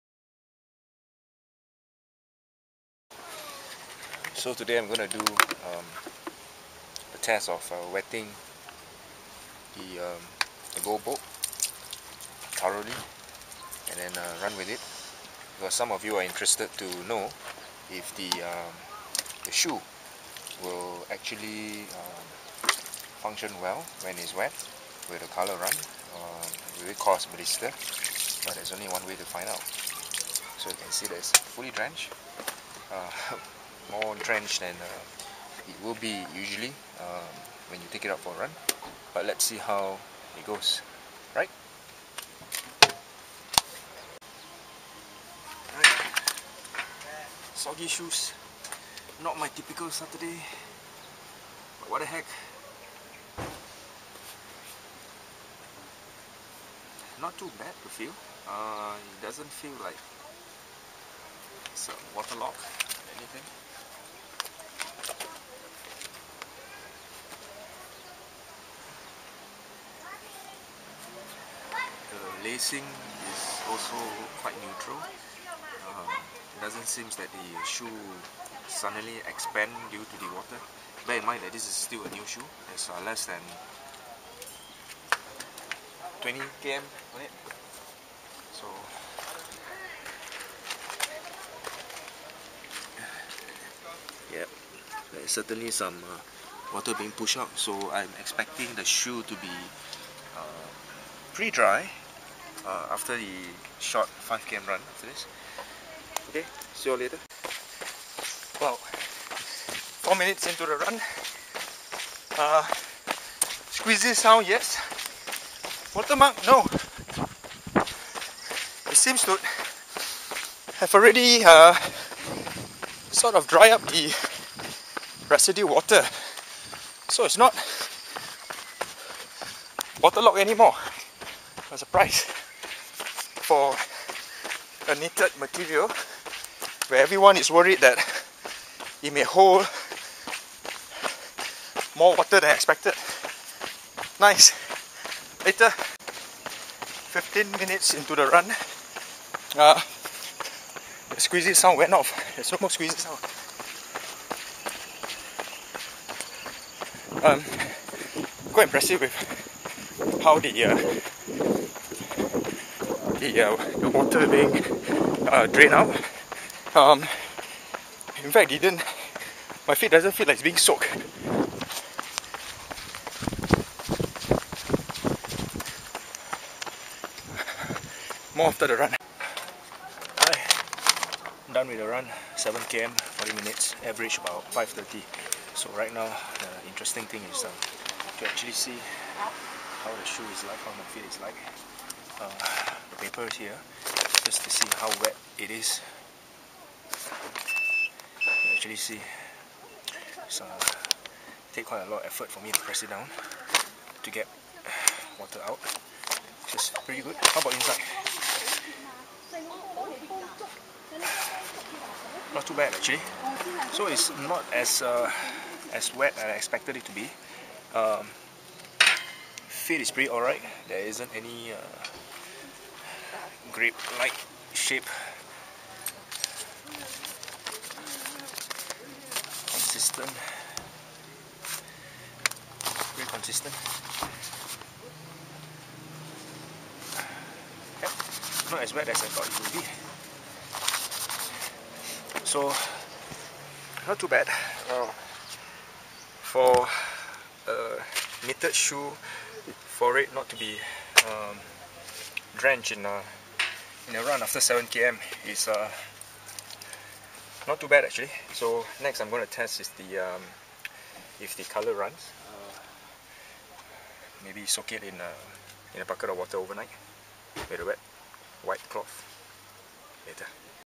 Skechers GoRun Bolt Fitknit Soaked Shoe Run
Wet the GoRun Bolt with 1.5liters of water and then ran in it as there were request by friends to test them out when it is wet (like during a race).